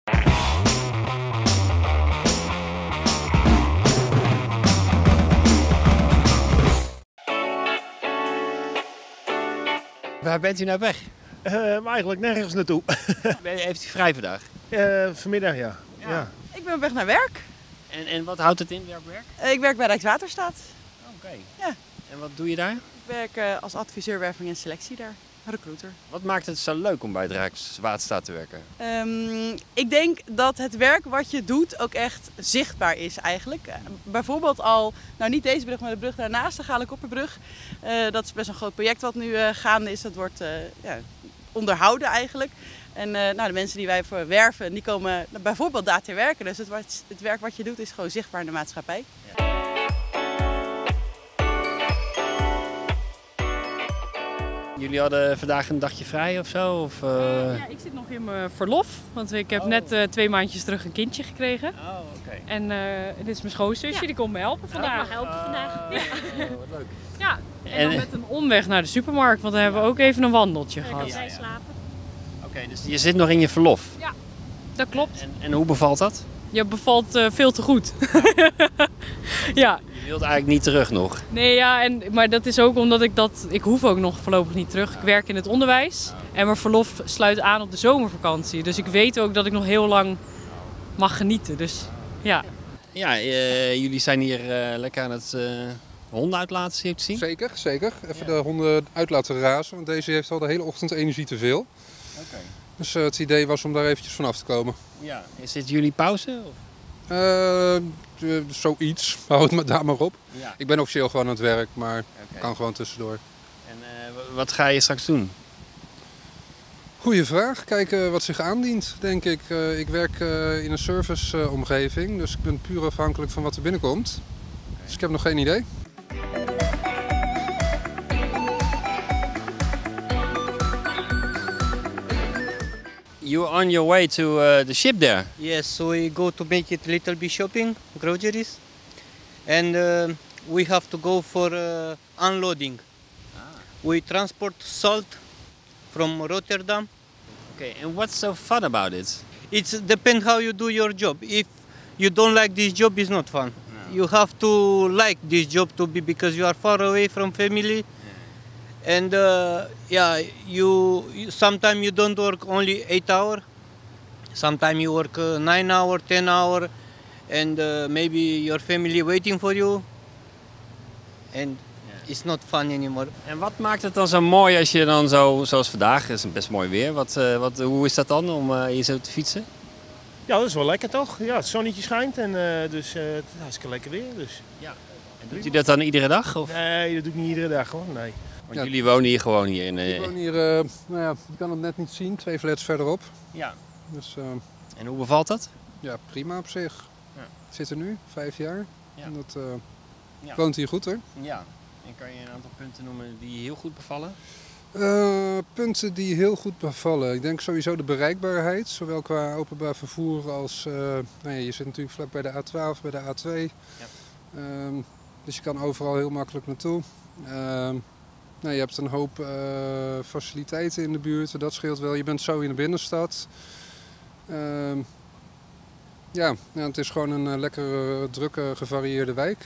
Langs het Kanaal
Ed Spil trok met de camera naar het kanaal en sprak met voorbijgangers: Qua vadis? Waarheen leidt uw weg? Omtrent zout, de harde werkelijkheid van de binnenvaart en werk dat zichtbaar is voor de maatschappij.